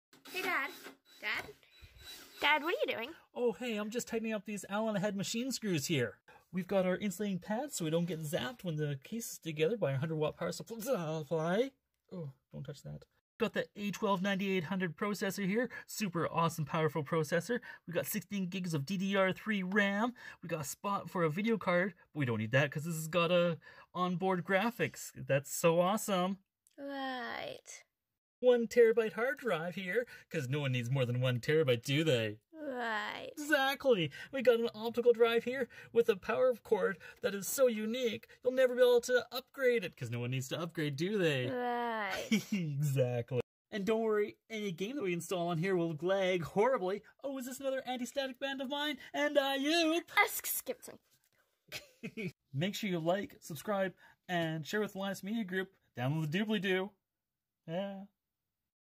ASUS ROG RIG Reboot 2019!!
Our submission to the ARRR, and this happened, because, yes, it's a horid computer we got sucked into buying, and I lost half my brain cells, making fun of Verge, and going "And I Oop"